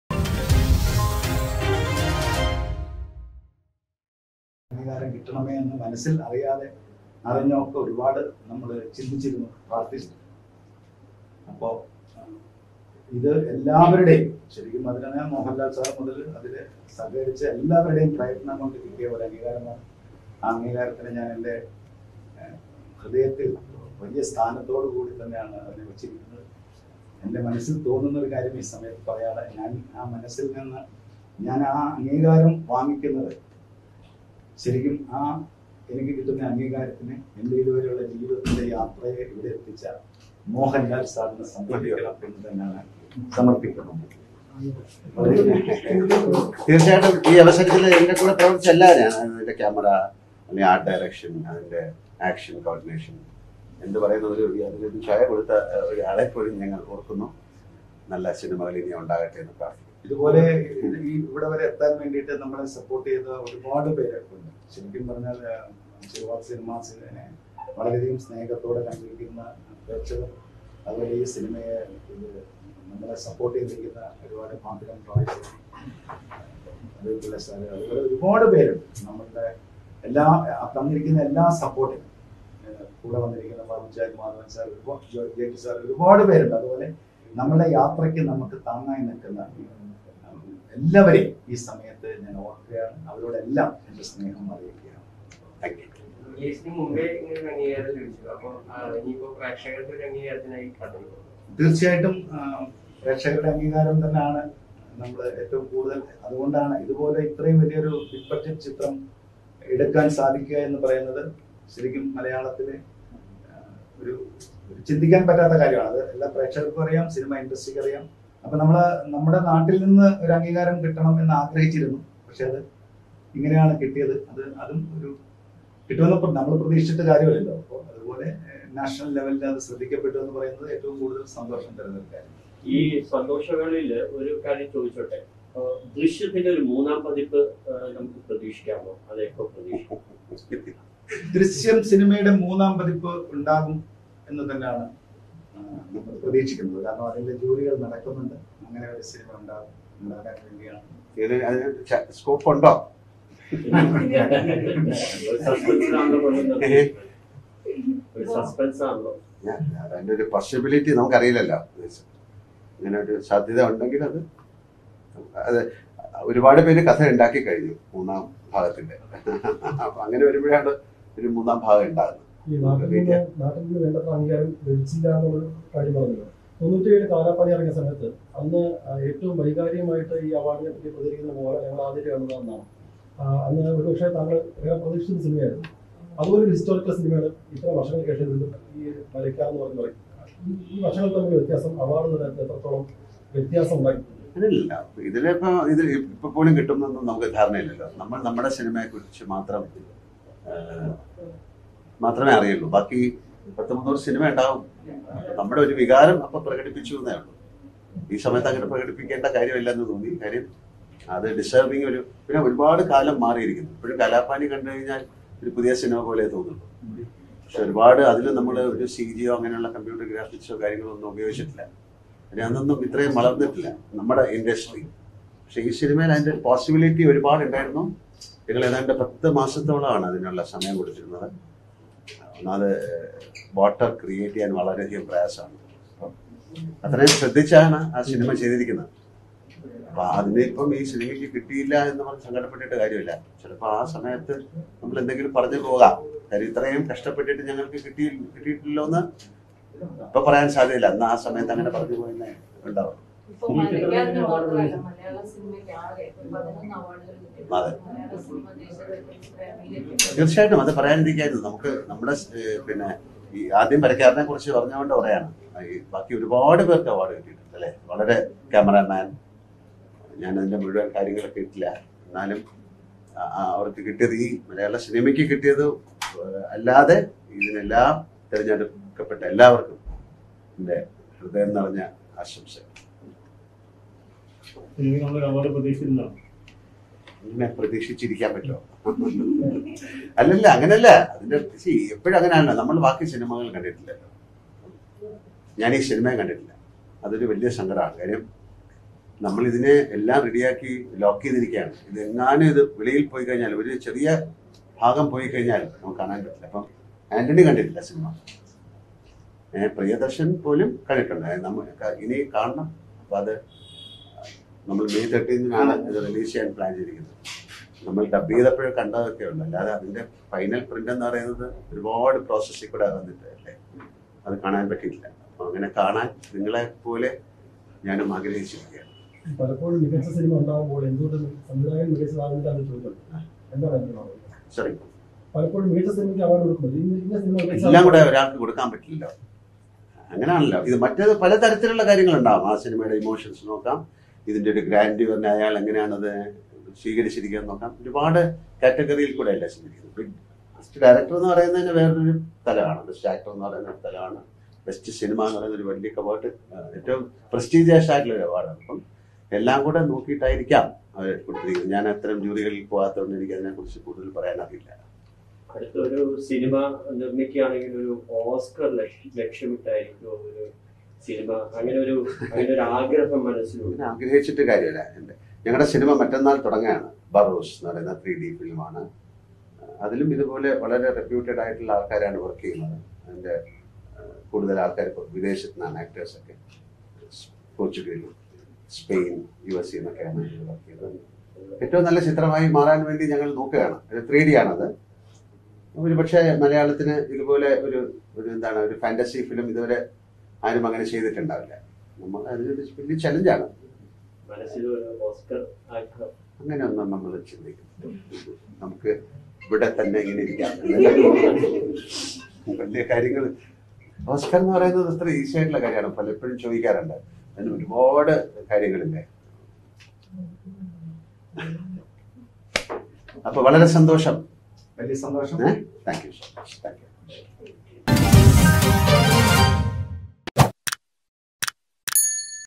Mohanlal's reaction to the selection of "Maraikkar" as the best national film
" മരയ്ക്കാർ " മികച്ച ദേശീയ ചിത്രമായി തെരഞ്ഞെടുത്തിതിൽ മോഹൻലാലിൻറെ  പ്രതികരണം